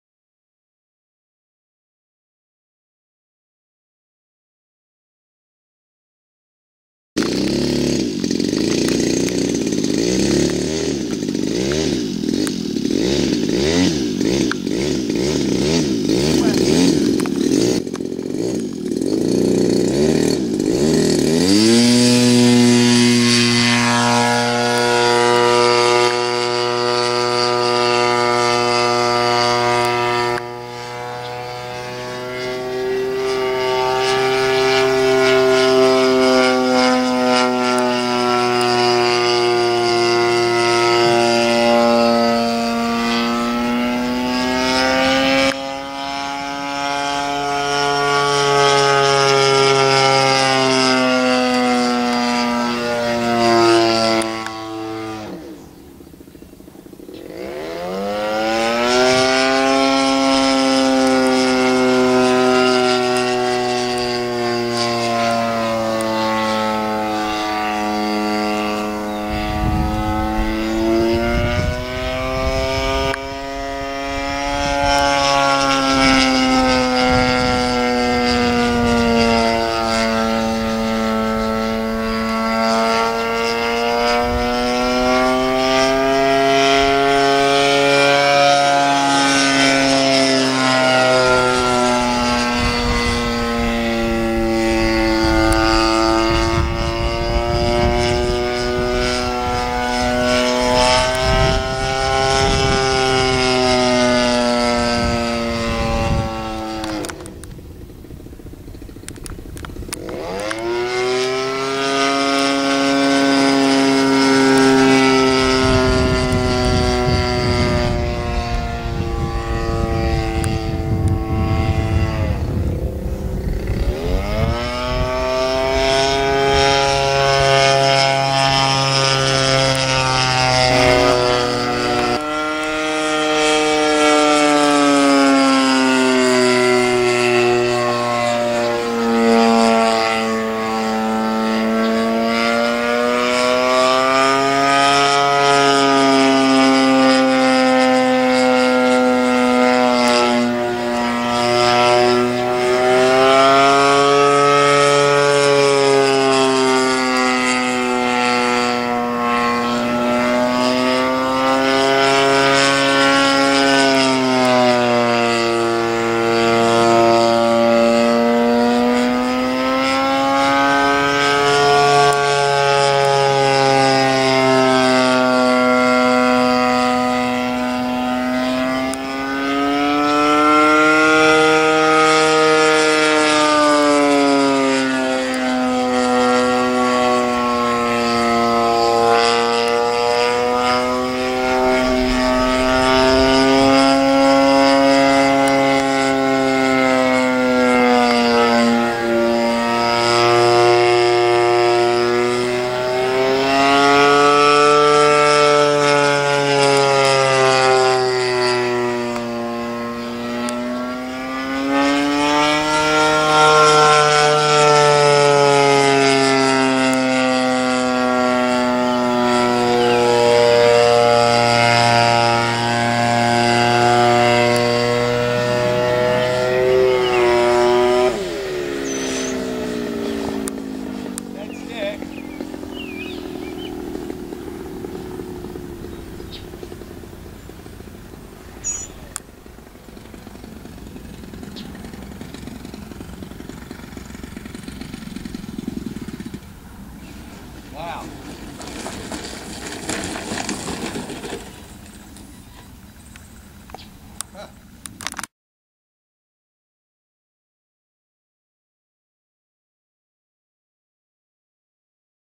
At AFRCF______ Tom's Stearman 7 15 12
Tom Flying his Stearman at A.F.R.C.F. Landing with a broken throttle linkage.